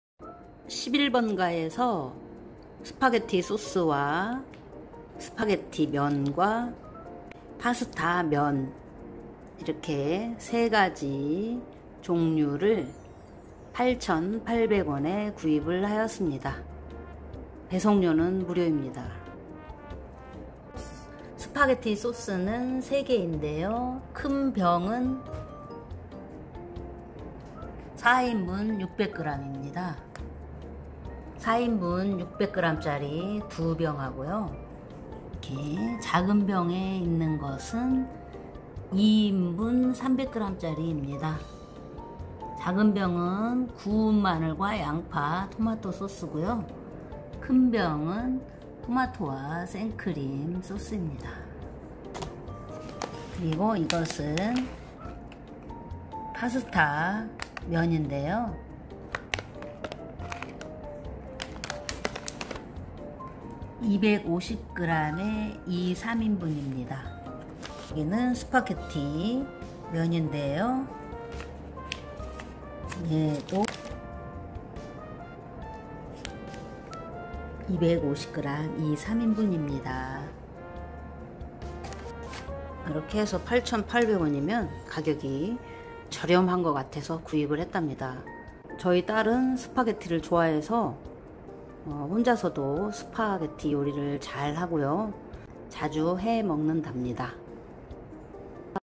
스파게티 소스와 면을 샀어요♡
2021.8.2.월요일

11번가에서 딸이 스파게티 소스와
면을 샀어요.

청정원
스파게티 소스 600g 4인분 2병
스파게티 소스 300g  2인분 1병
파스타면 250g 2-3인분 1봉지
스파게티면 2-3인분 250g 2봉지
= 8.800원

배송비 무료